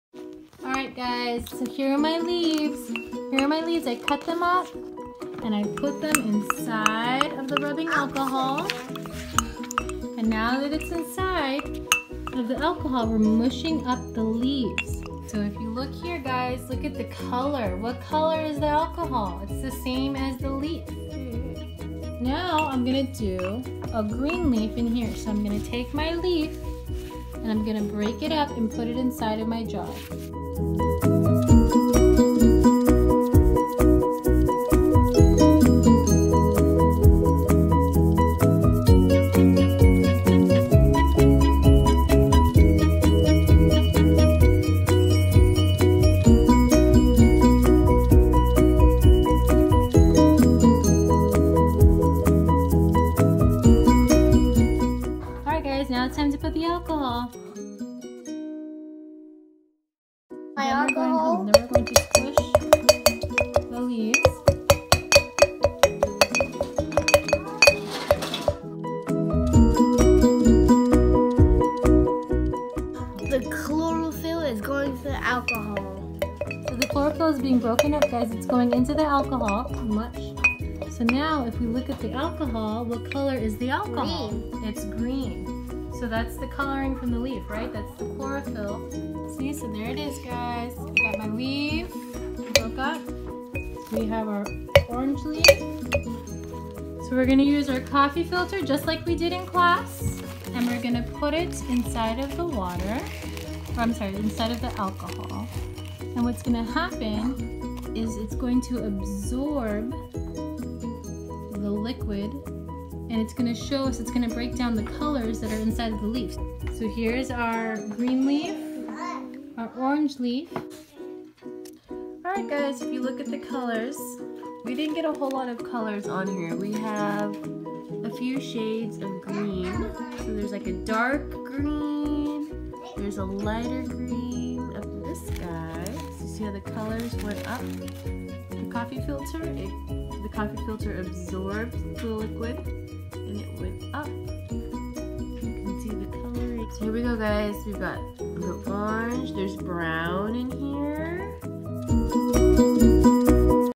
HOW DO LEAVES CHANGE COLORS? SCIENCE EXPERIMENT
Preschool science project. Leaves contain Chlorophyll.  Chlorophyll makes the leaves green and is so dominant that it covers up all the other colours in the leaves. To figure out what colour a leaf would be without the dominant chlorophyll colour, we can separate the colours by doing this science experiment.